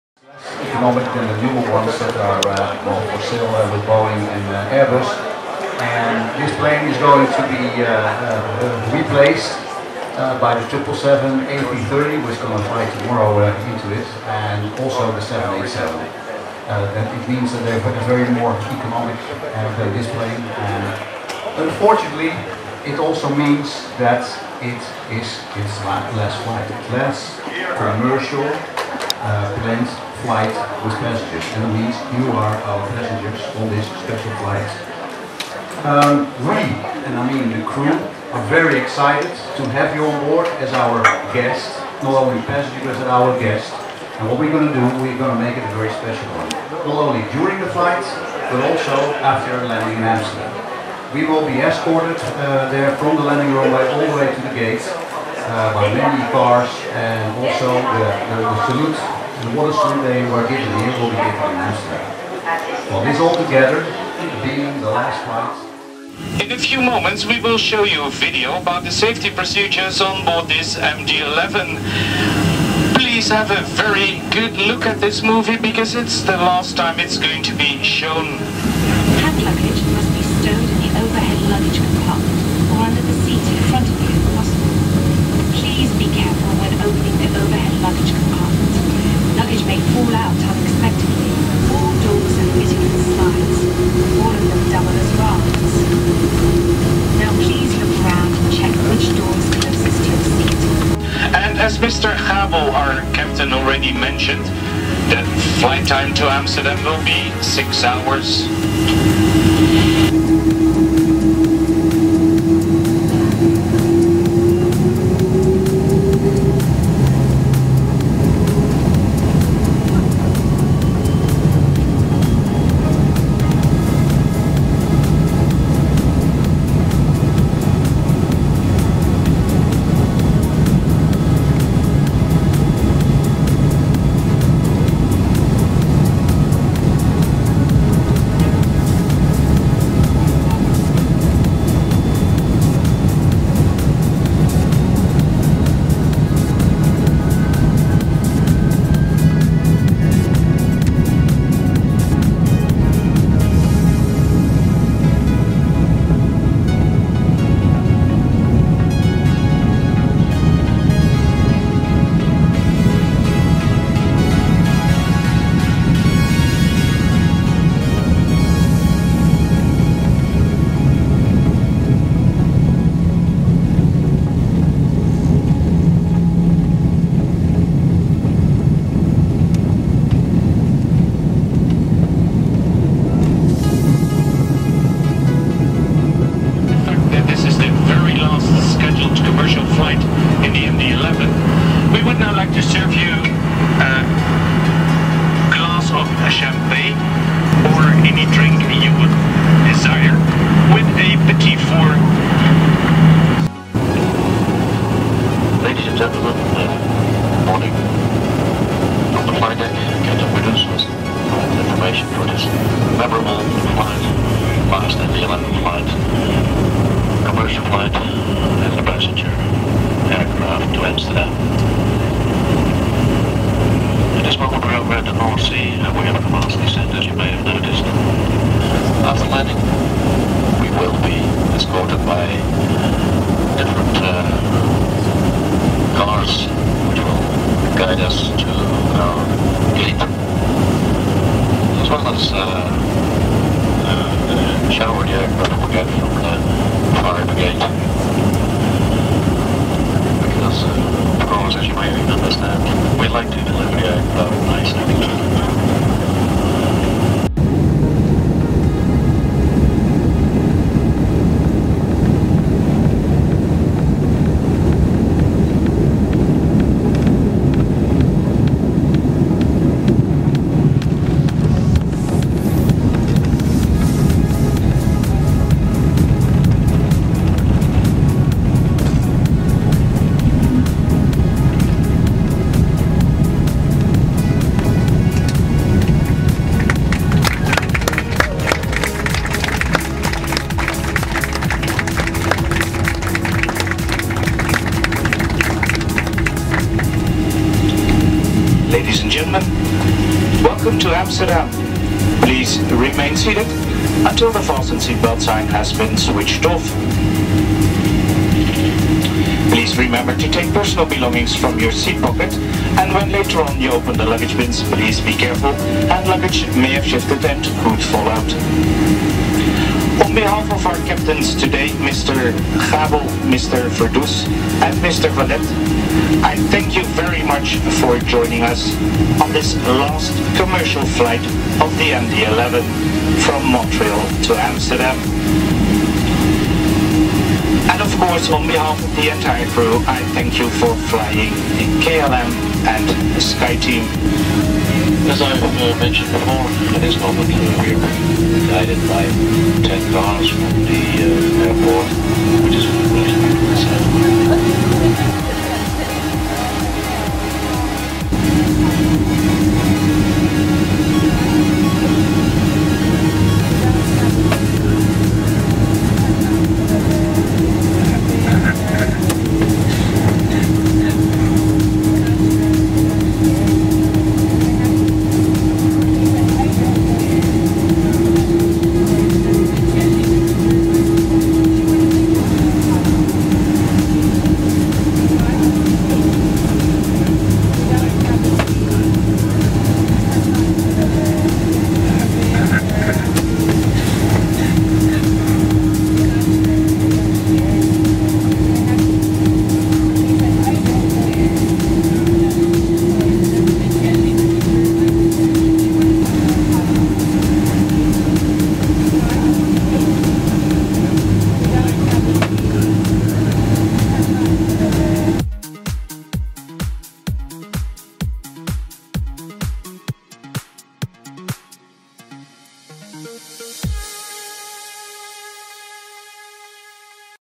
Farewell flight KLM MD-11
The farewell flight of the last scheduled passenger MD-11 flight operated by KLM on October 25th, 2014 from Montreal to Amsterdam as KLM672. The flight was operated by PH-KCE named after Audrey Hepburn which also has the 95 anniversary sticker for KLM operations.